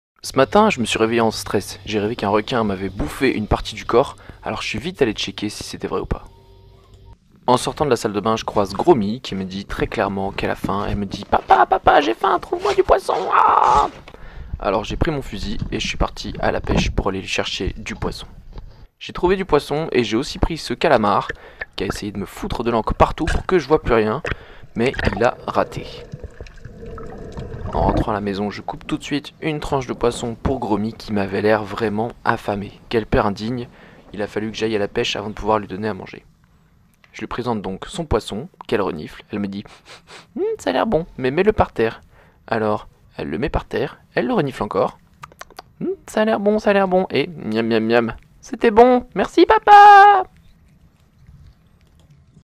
Je pars pêcher pour mon chat 🐈
Surtout les gars, n'oubliez pas de nourrir vos animaux 😉